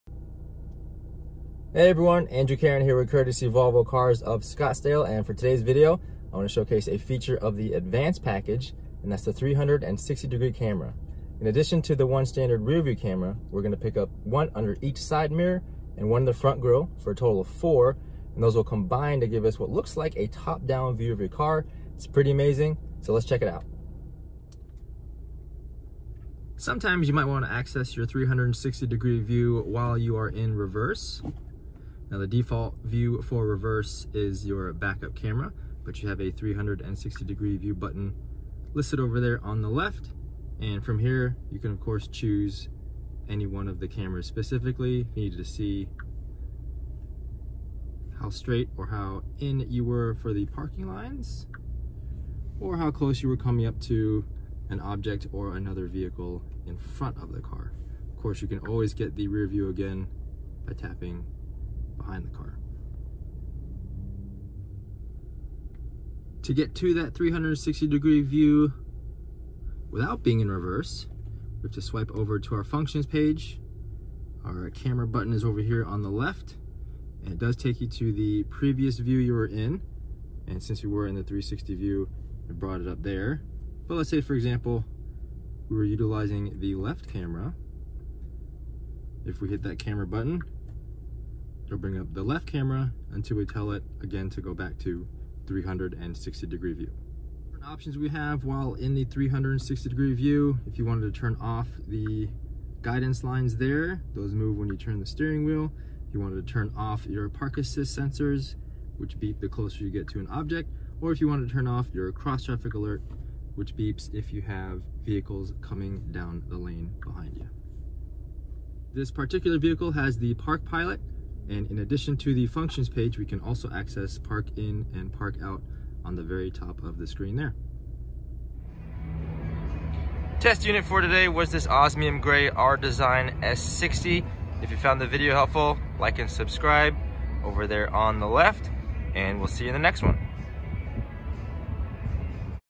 Volvo 360 Camera
How to use Volvo 360 Camera. Andrew Caron showing how the advance package upgrades the cameras around your Volvo to give a 360 view of your Volvo.

Or